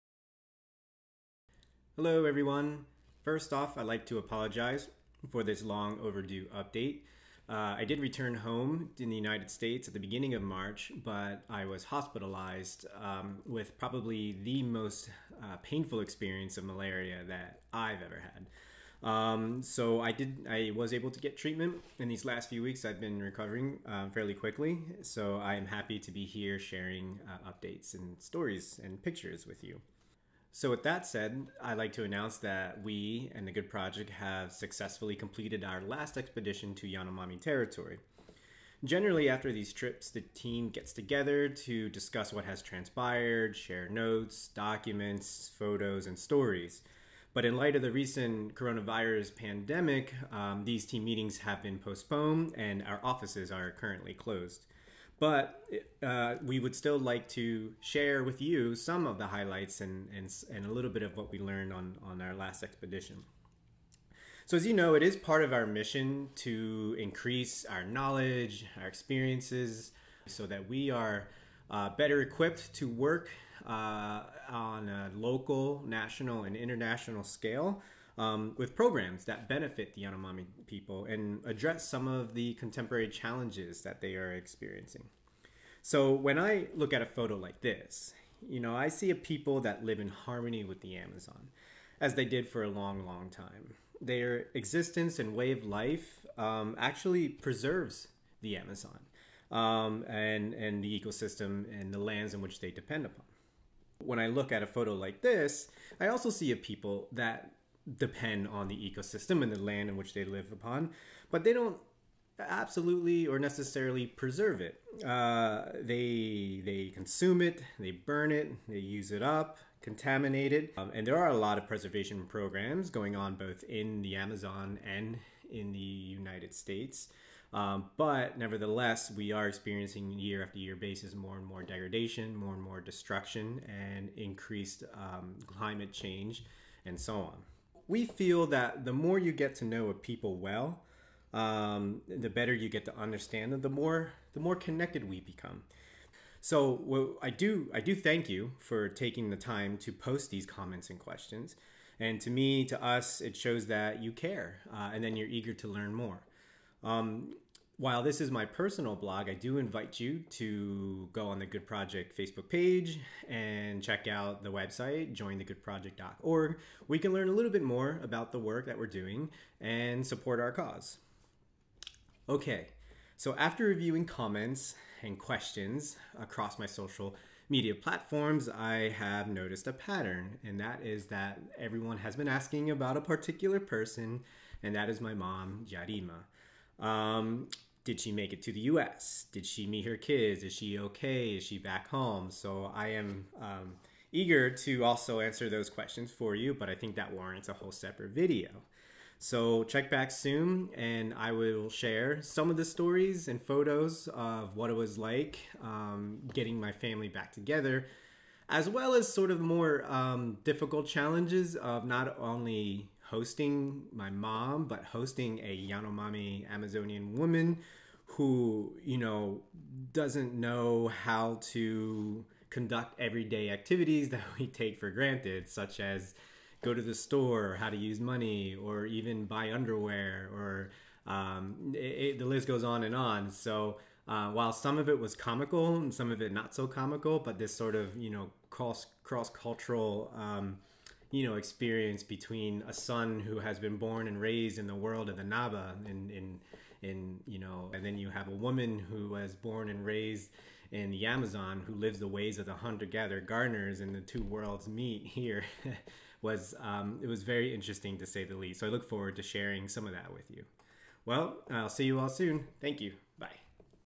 An update on my return from 2020 Expedition to Yanomami territory in the Amazon.
I recently returned to the United States from an amazing expedition with the Good Project. The upcoming videos will touch on Yarima's homecoming to the United States, the ever important microbiome research, projects in health and education, and of course, sharing the beautiful and amazing world of the Yanomami people.